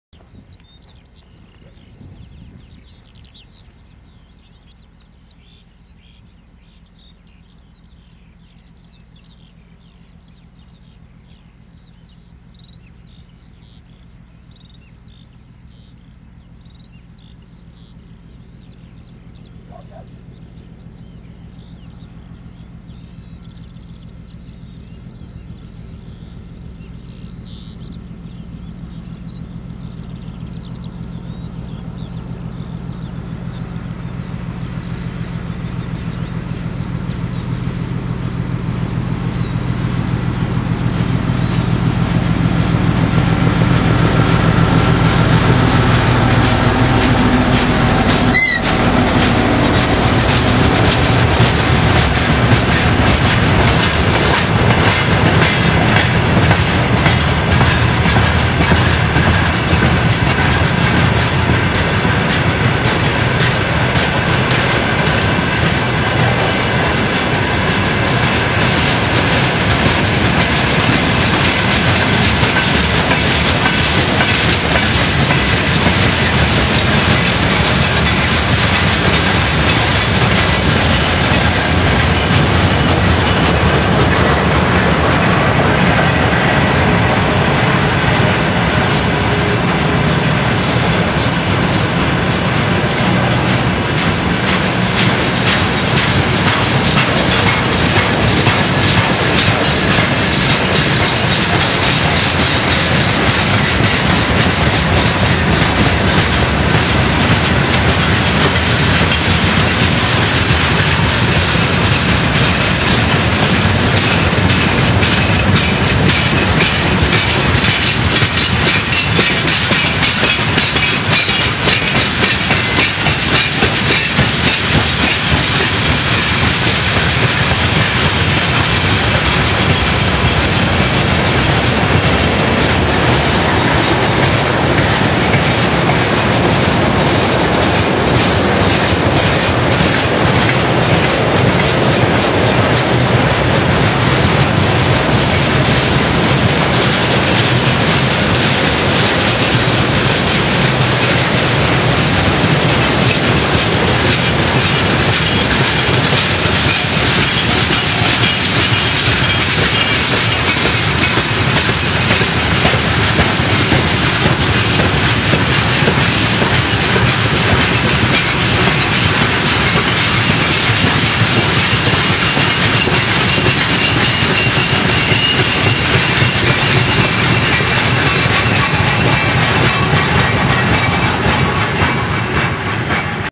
120408 M62-1707 Terespol-Małaszewicze
Cargo train from Belorussia to Poland. Course: Brest-Małaszewicze.Ummm...Nice sounds.